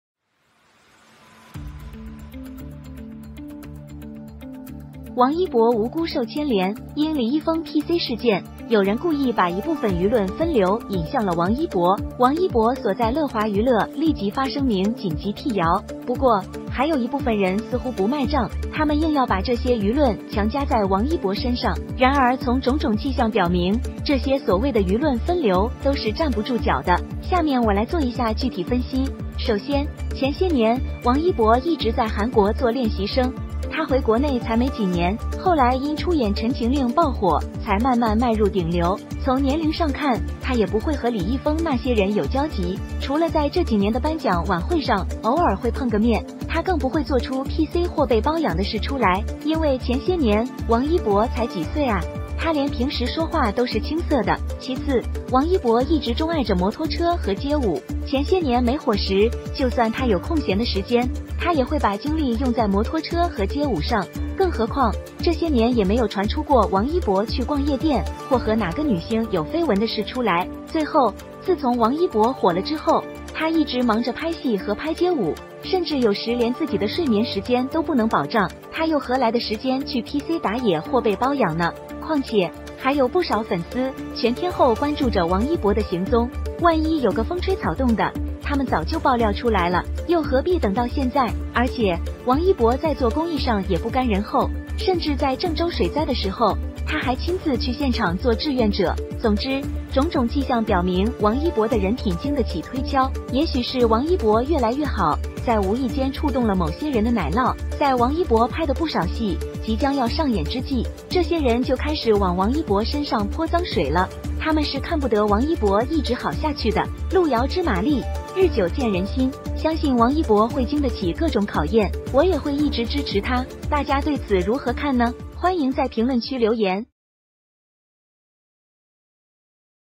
Wang Yibo was implicated innocently
Wang Yibo was innocently implicated! Because of the Li Yifeng PC incident, some people deliberately diverted part of the public opinion to Wang Yibo, and Wang Yibo's Lehua Entertainment immediately issued a statement to urgently dispel the rumors. However, there are still some people who don't seem to be sold on the idea and they insist on imposing these public opinions on Wang Yibo. However, from all indications these so-called public opinion diversions are untenable! I'll do some specific analysis below. First of all, some years ago, Wang Yibo has been working as a trainee in Korea, he returned to China only a few years. Later, because of the appearance of the "Chen Eros" burst fire only slowly into the top stream. From the age he would not have crossed paths with those of Li Yifeng, except for the occasional encounter at the awards parties in the past few years. He will not even make PC or be adopted out, because some years ago Wang Yibo is only a few years old ah, he even usually speak is green. Secondly, Wang Yibo has always loved motorcycles and street dance. Some years ago when there was no fire even if he had free time, he would also spend his energy on motorcycles and street dance. What's more, over the years there have been no rumors of Wang Yibo going to nightclubs or having scandals with any actress. Finally, since Wang Yibo has been on fire, he has been busy filming and shooting street dance, and sometimes he can't even guarantee his sleep time, so how can he have time to go to PC to play wild or be adopted! Moreover, there are many fans who are concerned about Wang Yibo's whereabouts around the clock, in case there is a wind, they have already broken the news, and why wait until now. Moreover, Wang Yibo in doing public welfare is not willing to be left behind, even in Zhengzhou floods, he also personally went to the scene to do volunteer work. In short, all signs show that Wang Yibo's character can stand up to scrutiny. Perhaps Wang Yibo is getting better and better inadvertently touched the cheese of some people, in Wang Yibo shot a lot of drama is about to be staged, these people began to throw dirty water on Wang Yibo, they are not allowed to see Wang Yibo has been good! The road is far to know the horsepower, day long to see the hearts of the people. I believe that Wang Yibo will stand up to all kinds of tests, I will always support him! How do you think about this? Welcome to leave a message in the comments section!